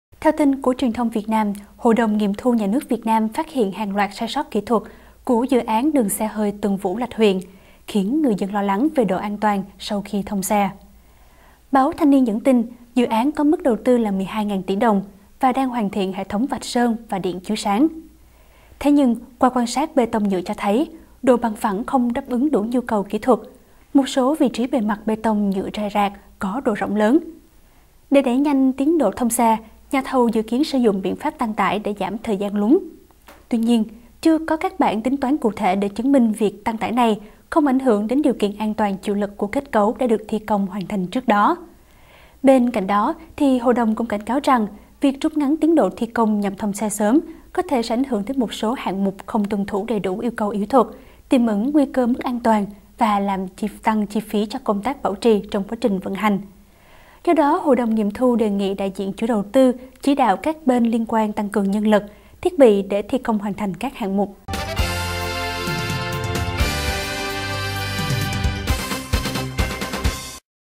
Dự án"cầu nối vượt biển dài nhất Việt Nam" đầy sai sót kỹ thuật
Theo tin của truyền thông Việt Nam, Hội đồng nghiệm thu Nhà nước Việt Nam phát hiện hàng loạt sai sót kỹ thuật của dự án “Đường xe hơi Tân Vũ – Lạch Huyện”, khiến người dân lo lắng về độ an toàn sau khi thông xe.